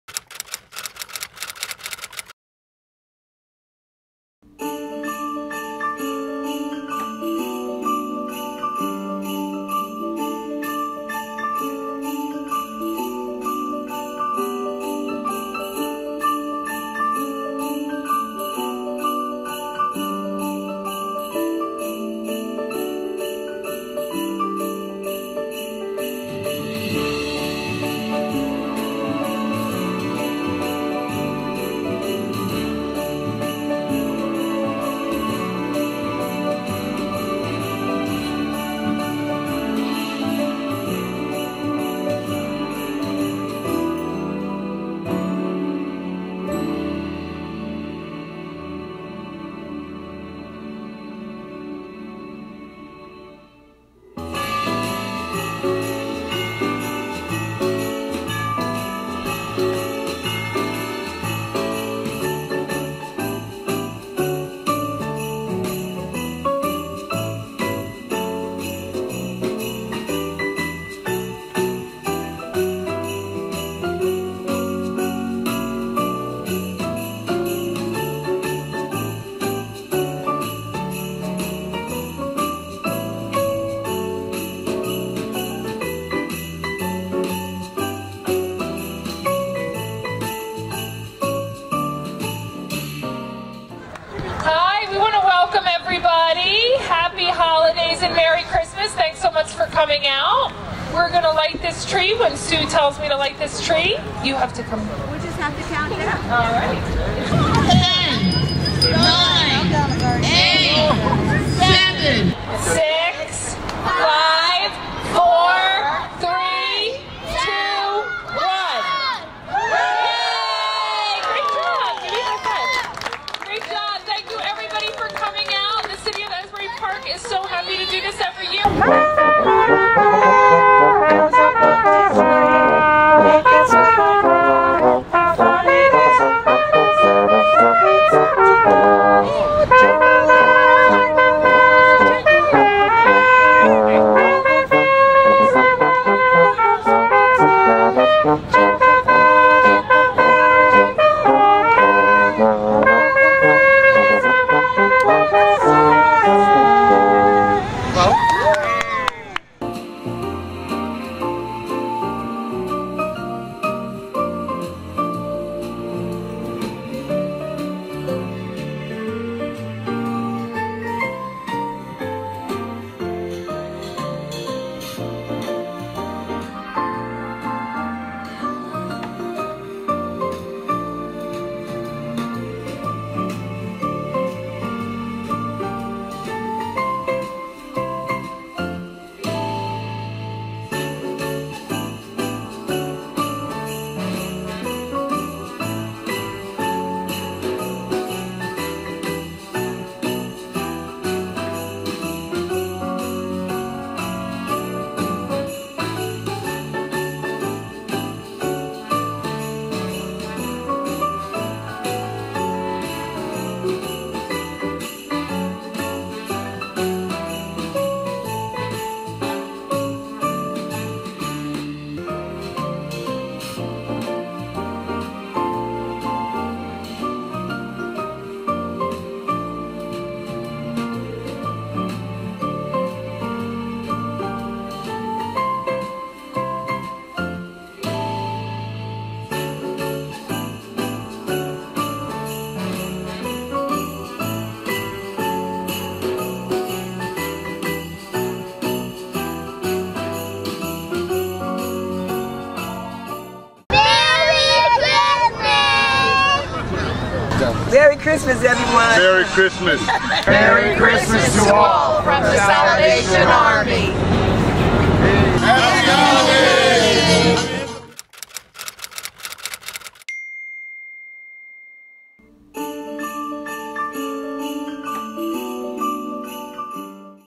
Asbury Park's 2018 Holiday Season
Highlights from Asbury Park's 2018 Fireman’s Park Tree Lighting, Kwanzaa Celebration and Springwood Park Tree Lighting.